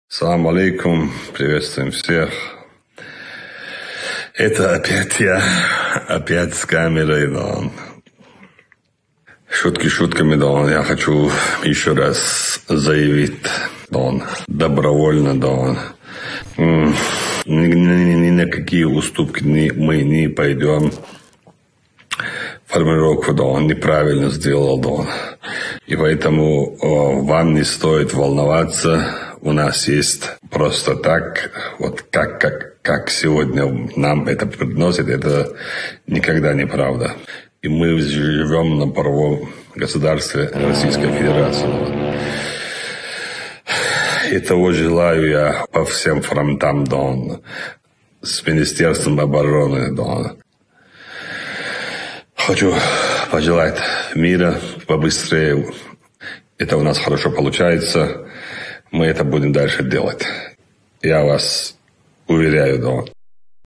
ЧЕЧНЯ 95: Рамзан Кадыров прибыл в Мариуполь, чтобы дать эксклюзивное интервью InformNapalm
Глава Чечни, генерал-лейтенант Рамзан Кадыров, находясь в Мариуполе, дал эксклюзивное интервью нашему сообществу.  ЧЕЧНЯ 95